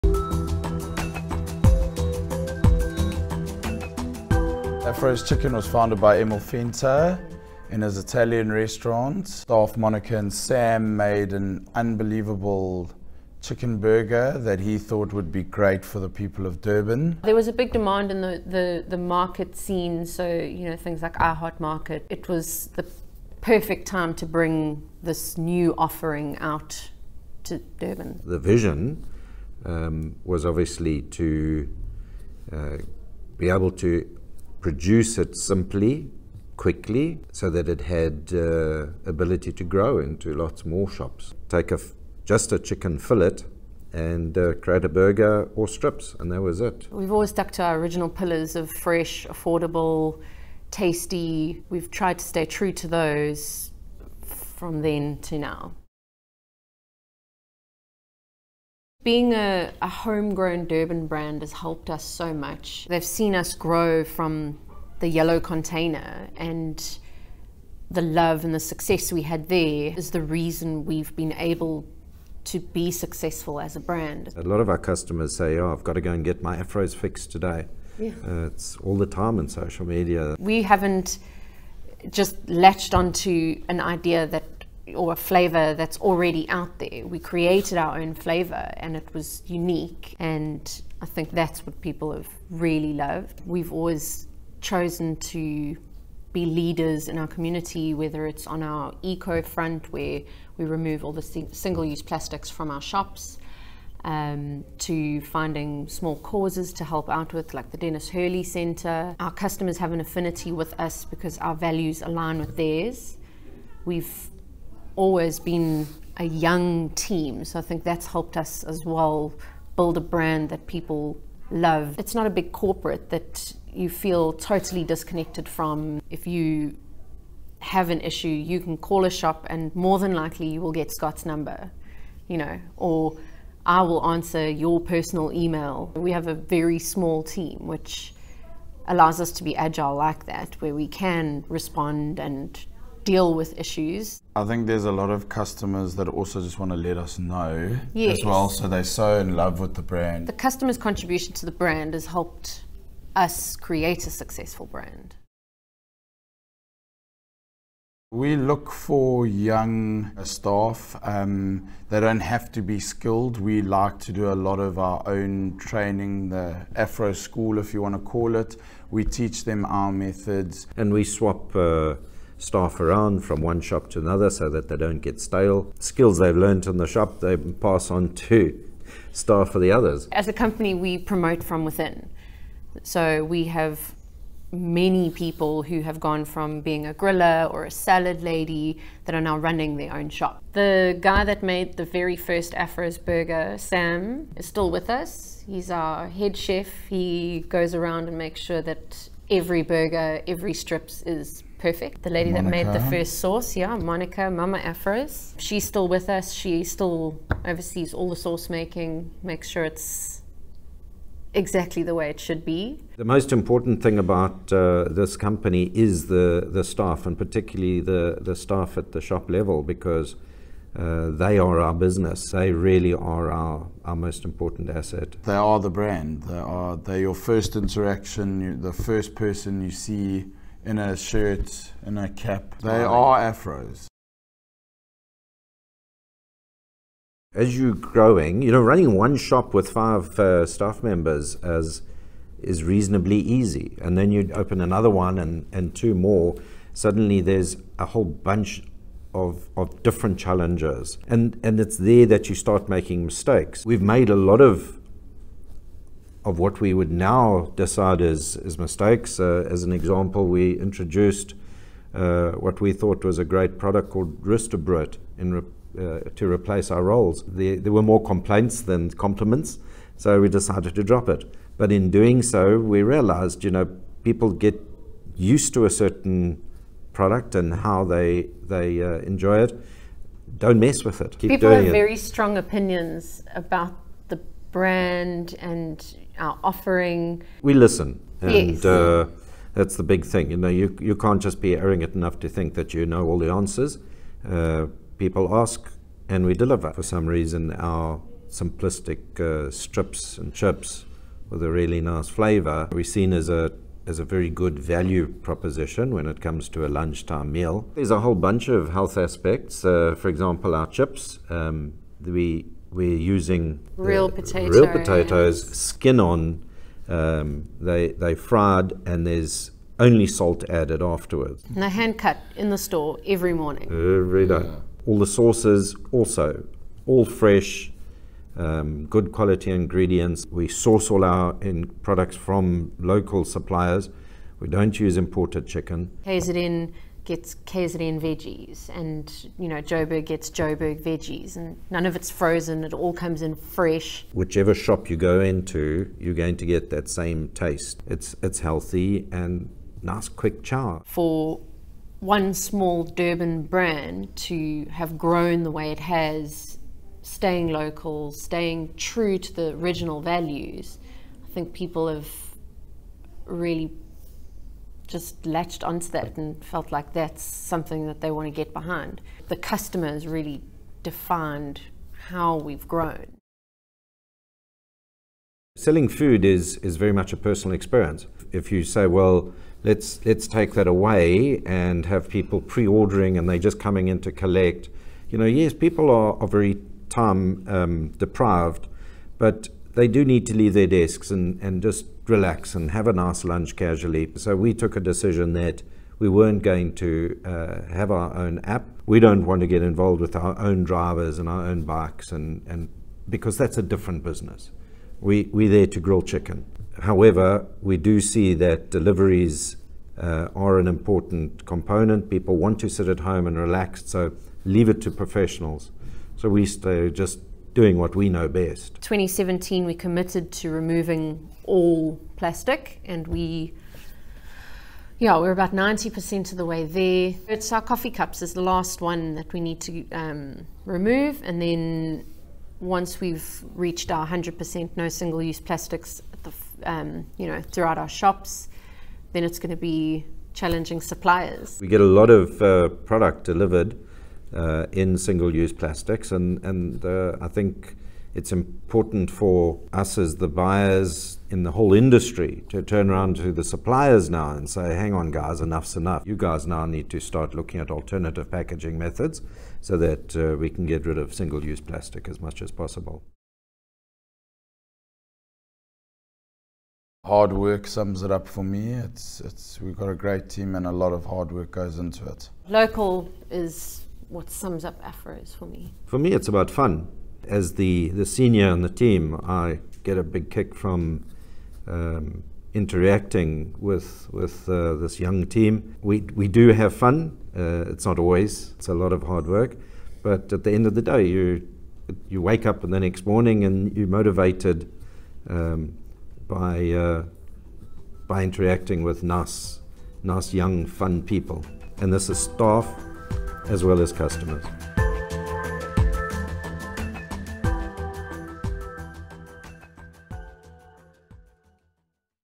Afro's Chicken - KZN Brand DNA
Afro's Chicken Shop is a homegrown food brand that has, until recently, been KZN's best kept secret. The brand was founded by restaurateur, Emil Venter, whose staff made an unbelievable chicken burger that he thought would be great for the Durban market. "At the time the trend was simple food that was fresh, tasty, healthy and affordable. The menu is uncluttered - take a chicken fillet and offer a burger or strips. The vision was to be able to produce meals simply and quickly, and to grow the brand into a lot more shops," says managing director, Mike Harris. The first Afro's Chicken Shop was in the distinctive yellow container on Durban's Addington Beach. Although the brand has since established shops in Gauteng and the Western Cape, our focus is still firmly on KZN as our birthplace and our largest source of revenue. "We believe that we are truly a KZN business; local is what sums up Afro's for me," says Dee Gravett, Head of Marketing. "This is a value that drives almost all key aspects of our business."

Afro's Chicken Shop employs youthful individuals at a head office level to maintain a culture of innovation and to attempt to have top-level management that is in touch with its target market. "While Mike Harris is the captain of the team, Afro's Chicken Shop is run by a small group of young leaders who all share the same passion and views," says, Scott Meston, Head of Operations. "We have a great team and a lot of hard work goes into running the business." "We've created a dynamic team who enjoy what they do and have vision beyond serving a meal," says Dee. All our decision making is made and executed by the same team. "The team is young and enthusiastic and does a great job getting things done," says Mike Harris, CEO. "I get a great kick about interacting with this team, they keep me young and on my toes. We do have fun, it is not always hard work." We believe in fresh, simple food  made well with the best ingredients.

Our medium-term plans going forward are to open five more sites in the eThekwini region and three more sites in the broader KZN region. In Gauteng, we aim to complete two sites this year and plan for another three sites in 2020. In the Western Cape, we plan for another two sites in 2020. Although our growth rate appears small in the context of other food chains, we have taken the approach of owning all our shops and not franchising. "This is so that we may exert greater control in the direction of the brand as well as the in-shop quality provided to customers," explains Scott. "Our customers have a love for the brand, which has enabled us to be successful, spread our wings and grow from the yellow container," concluded Dee.